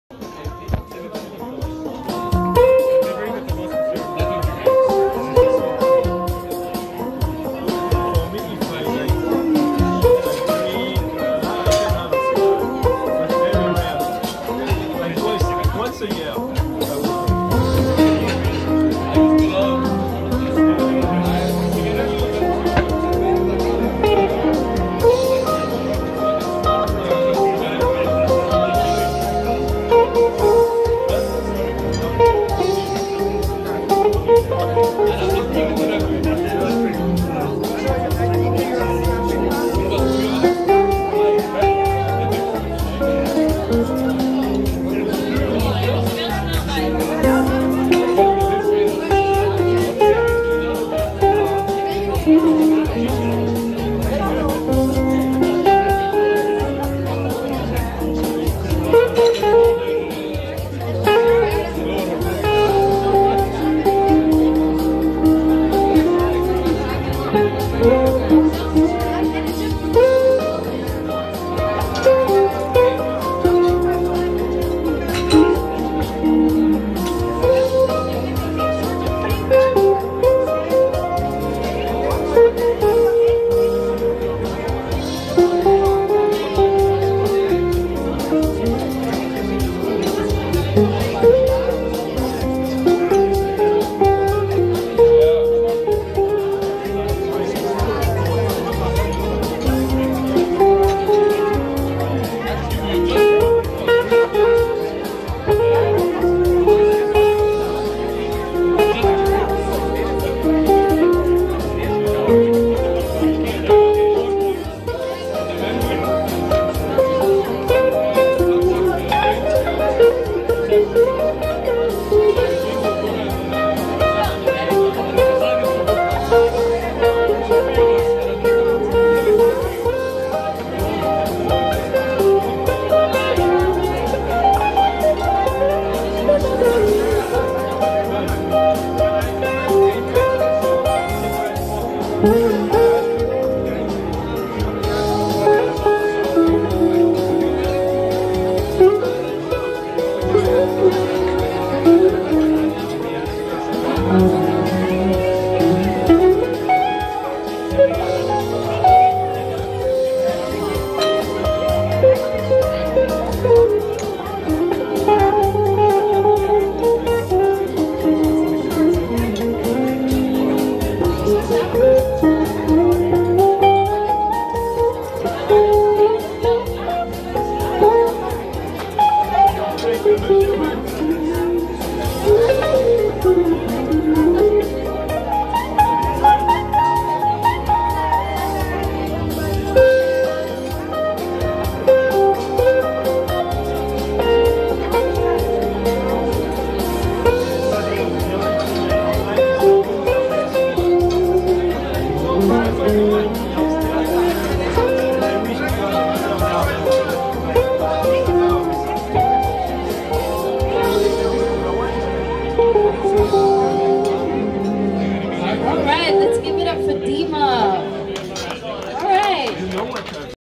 Simply Stunning UK Female Soulful Pop Singer in Dubai with Ace Guitarist
wdv0wefdg4eth4ethedfsd
X7UP